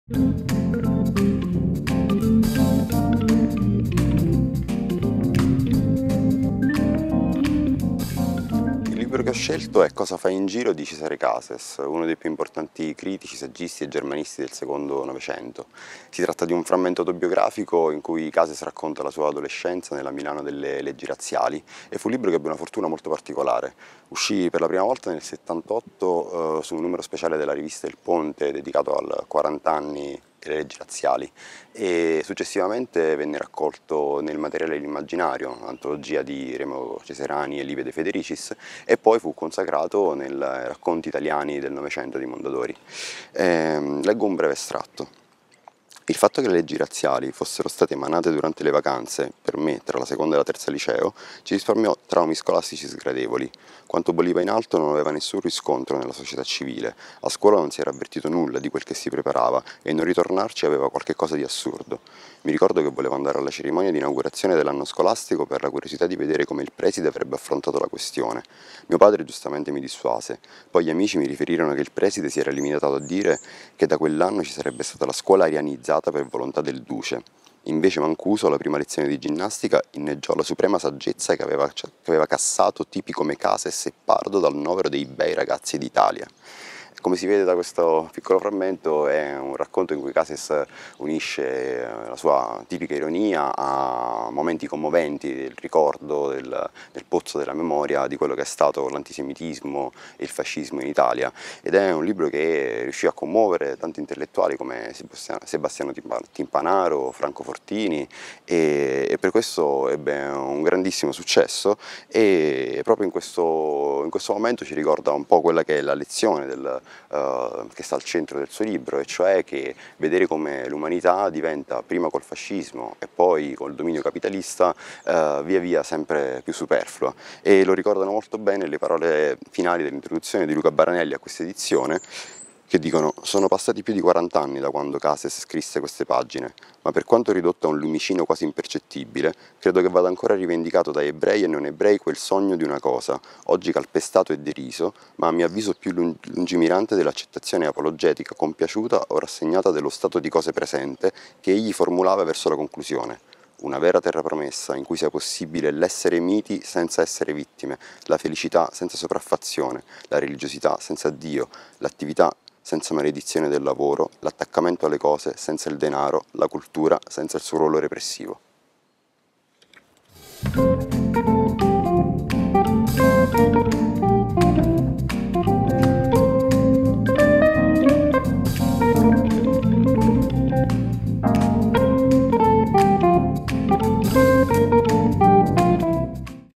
…in 2 minuti – Professori, studiosi e appassionati raccontano un libro (20° appuntamento)
La Biblioteca di area umanistica dell’Università di Siena presenta: “...in 2 minuti”
Un invito, un consiglio, un'occasione di riflessione, promozione e condivisione della lettura in un booktrailer di due minuti. Professori, studiosi e appassionati raccontano un libro scelto tra i ricordi, i grandi classici, lo studio e la ricerca o le proprie preferenze.
Il progetto aderisce al "Patto regionale per la lettura" della Regione Toscana, sottoscritto dall'Università di Siena.

Questa settimana abbiamo il piacere di condividere il booktrailer realizzato da
Sabatino Peluso
che ci parla del libro
“Cosa fai in giro?” di Cesare Cases 📔
Potrai trovare quest'opera in biblioteca alla collocazione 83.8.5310